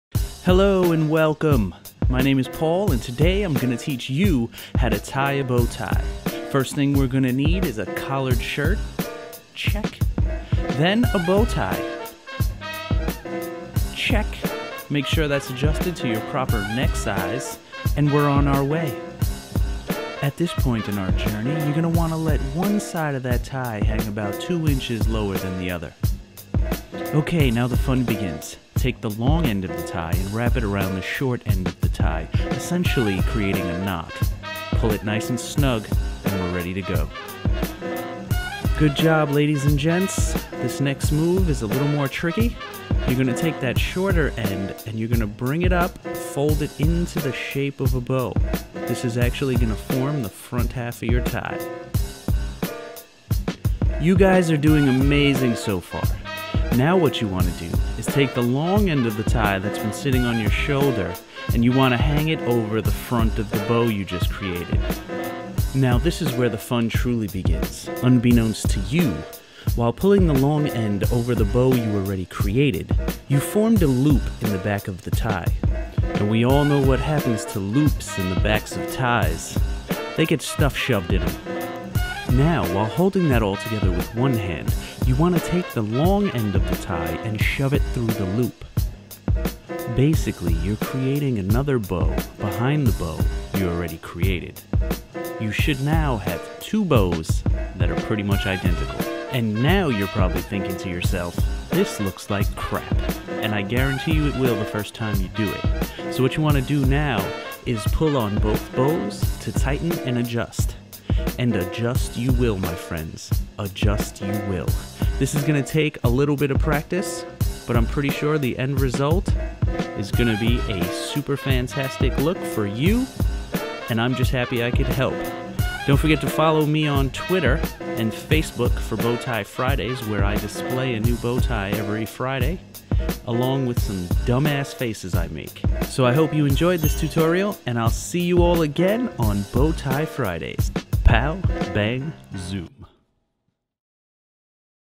How To Tie A Bow Tie
Everytime I wear a bow tie I hear two things from people. I like your tie, and I wish I knew how to tie one. I usually just reply with a thank you and it's not that hard (unless their really hot) but now I can say, go check out my video and tie one on.

I hope you found it helpful, if not, I hope you found it a slightly entertaining.

Check me out every friday for a new BowTie Fridays pic on Twitter and Facebook.